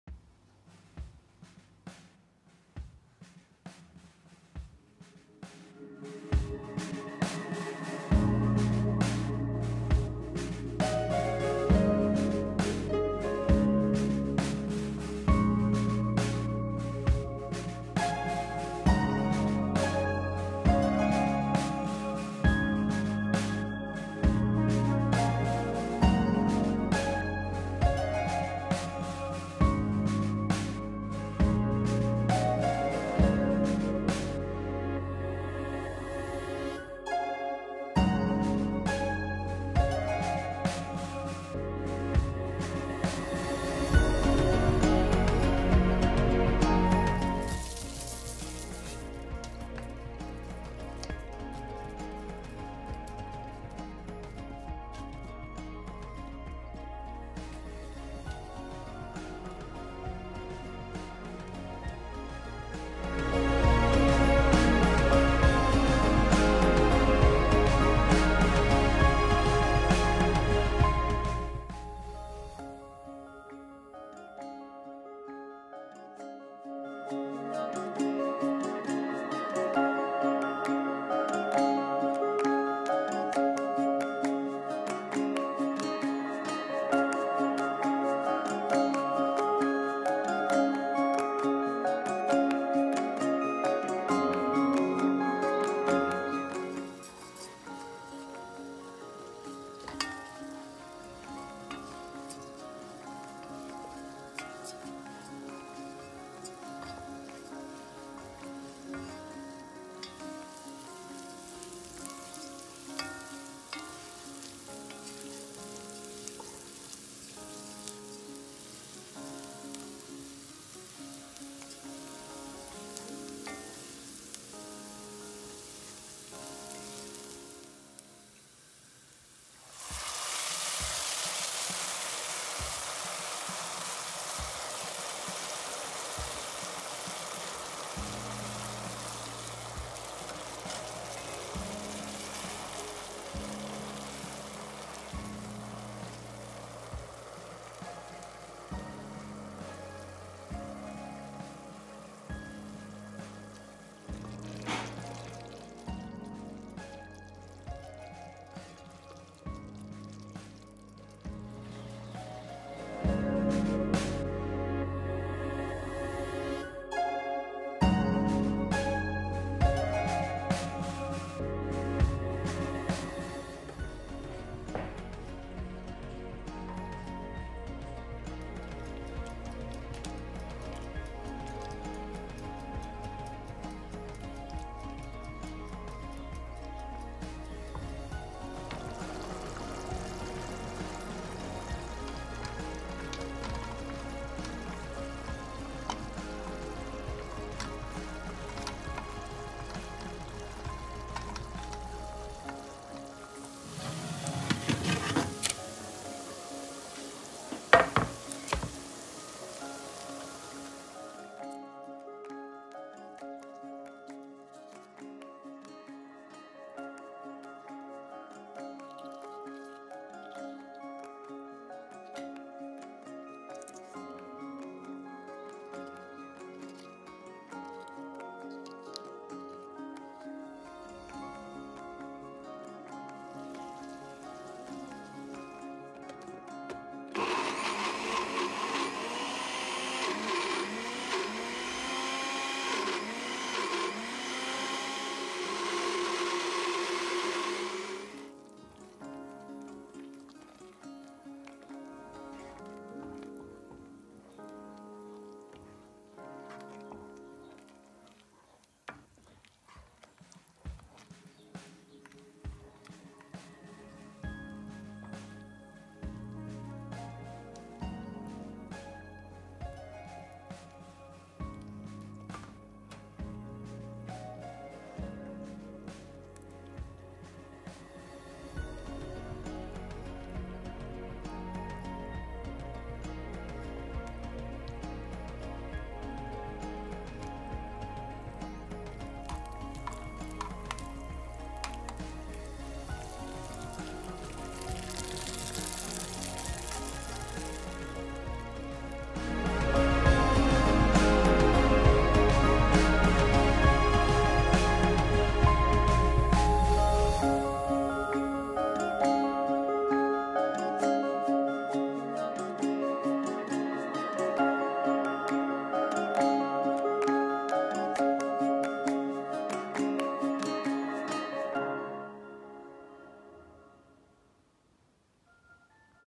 Rizoto sa bundevom (tikvom)
Koristila sam: 250 g princa (rize za rizoto), pola bundeve (hokkaido), 2 manje glavice crnog luka, 3 cena (veca) belog luka, 2 kocke bujona od povrca, 1 l vode (mozda zatreba malo vise ili malo manje), 4-5 kasika maslinovog ulja, 50 g lesnika, 1 grancica ruzmerina, 1 kockica putera (biljnog za posnu varijantu), so i biber po ukusu